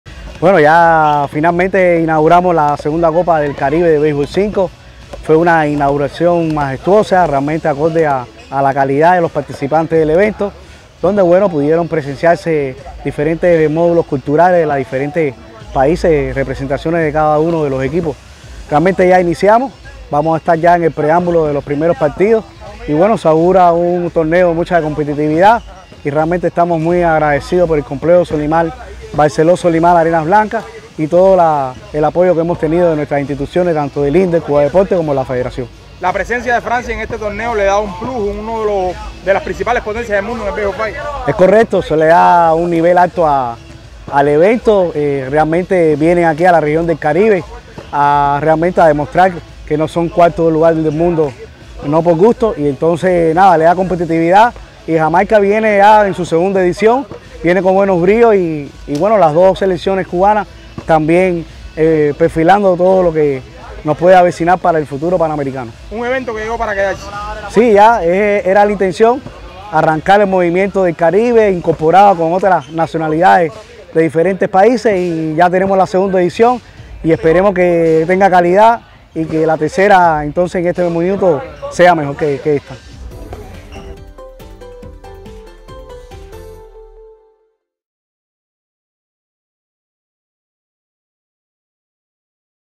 Se inició este lunes en Varadero Copa del Caribe de Baseball5
La Copa del Caribe de Baseball5 inició este 3 de junio en el Balneario de Varadero, Matanzas, desde el lugar nuestro periodista Duanys Hernández nos trae los detalles.